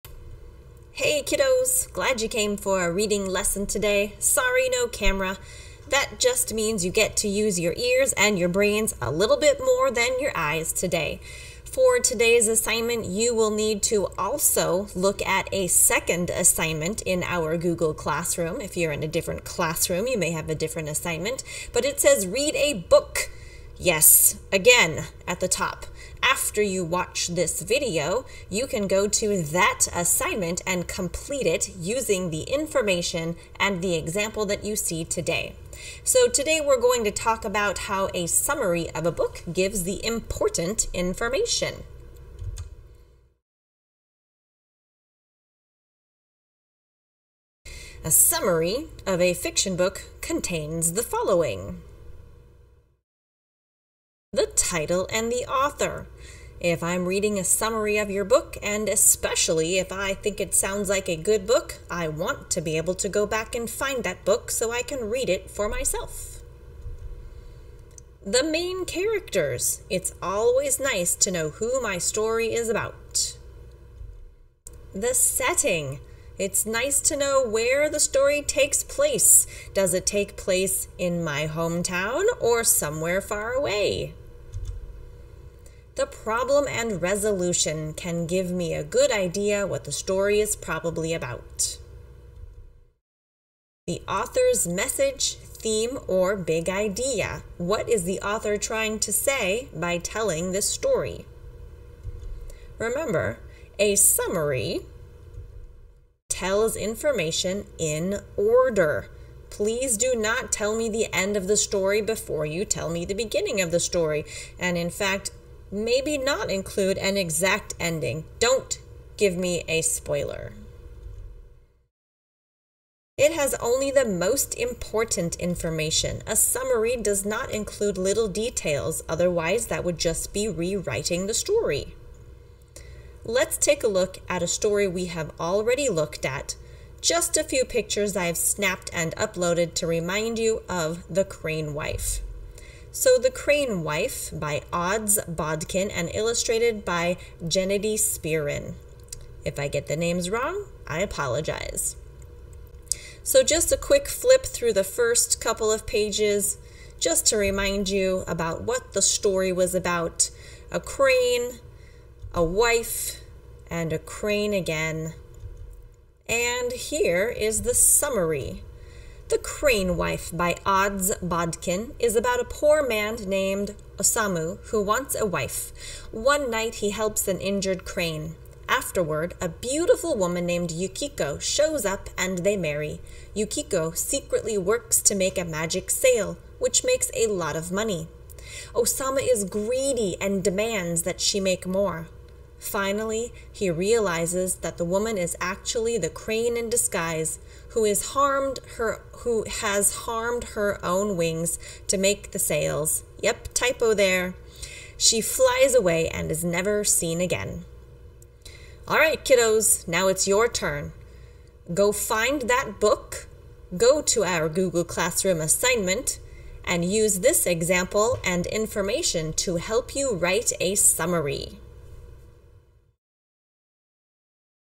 A summary contains
Ms. E. talks about what a summary should have and gives an example.